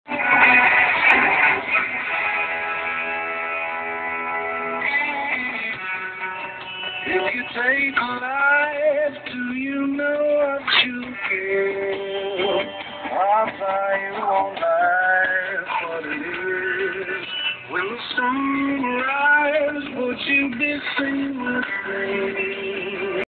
Classic American, pulling a trailer
I was driving up to mohegan sun, and thought that this Car should not be pulling a trailer... It just looks so wrong!